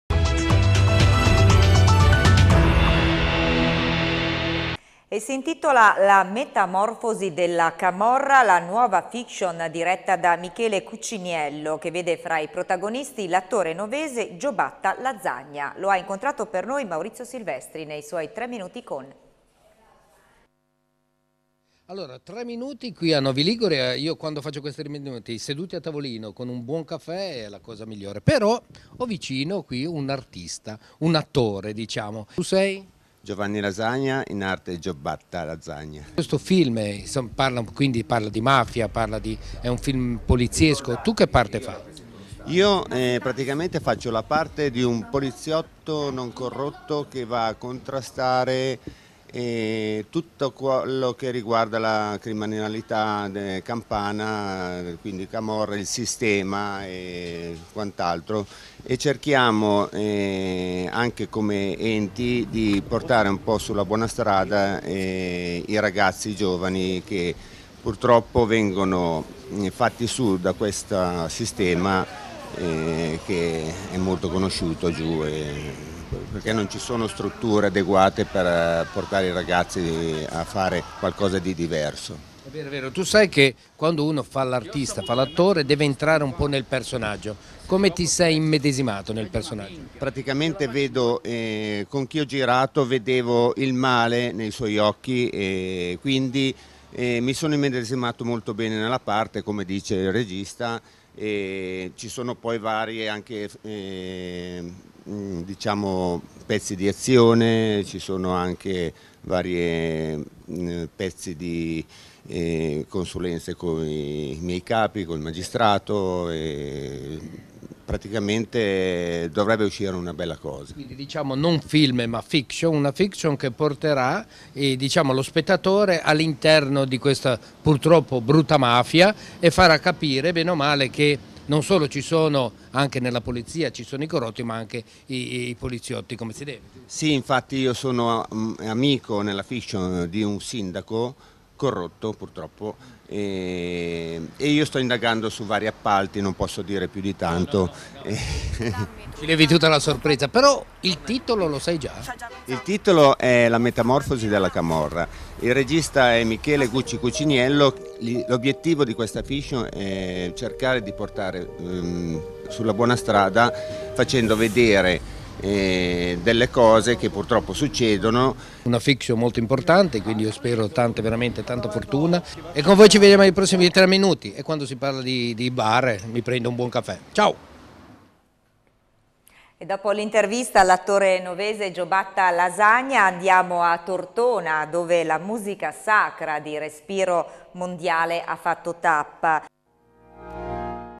GOLD7 : intervista 3 minuti con Giobatta Lazagna
Telecity Gold7
Spot intervista a Giobatta lazagna sulla fiction di Michele Cucciniello &quot;LA METAMORFOSI DELLA CAMORRA&quot;
QUESTA SERA ORE 20'30
...SEGUIRA' A MAGGIO PUNTATA COL CAST COMPLETO